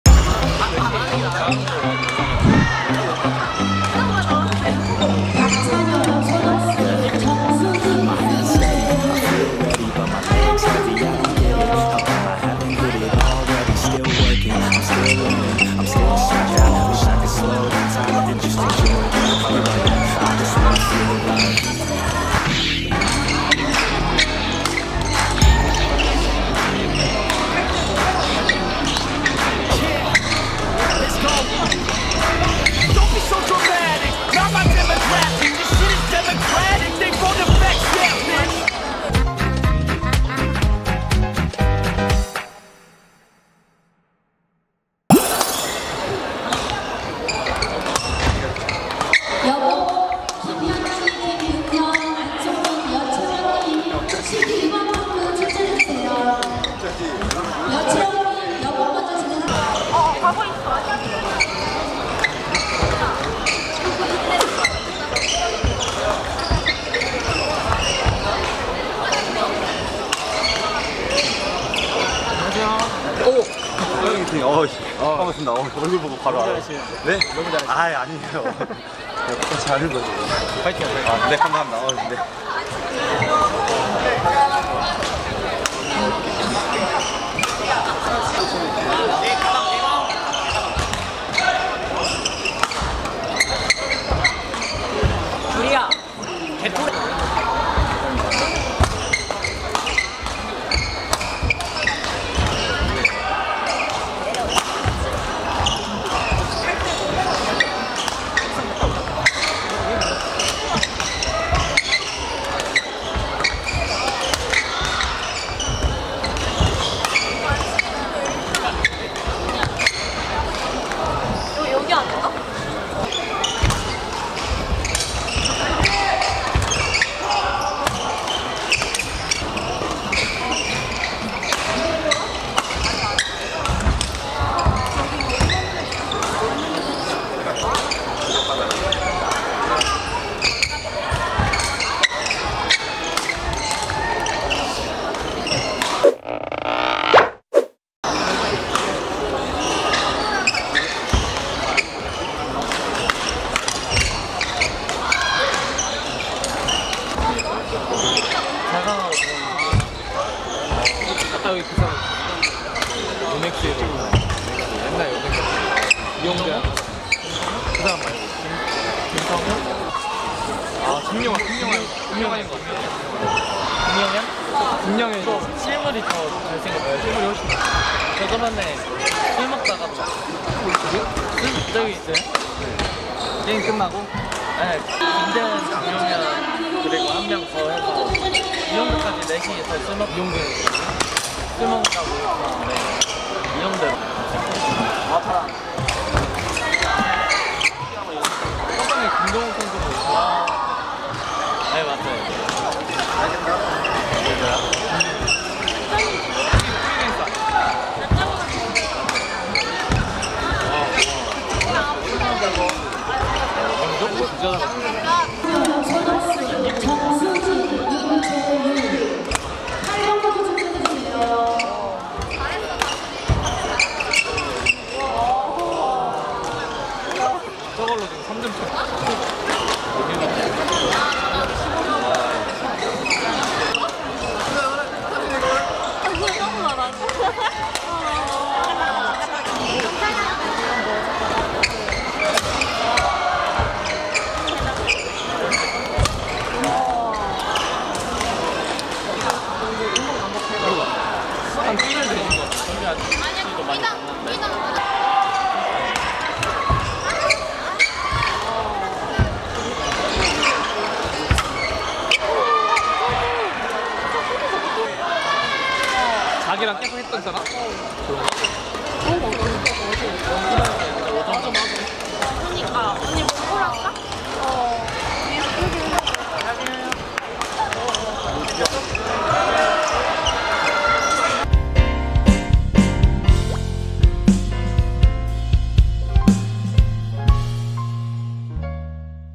배드민턴 자강 단식 예선전 임승기vs손유진 (쏘텍스컵 통일로 스포츠연맹 전국 배드민턴 대회!)
✅대회: 쏘텍스컵 통일로 스포츠연맹 전국 배드민턴 대회

✅급수: 자강

✅선수: 손유진vs임승기

🏸편집:순삭이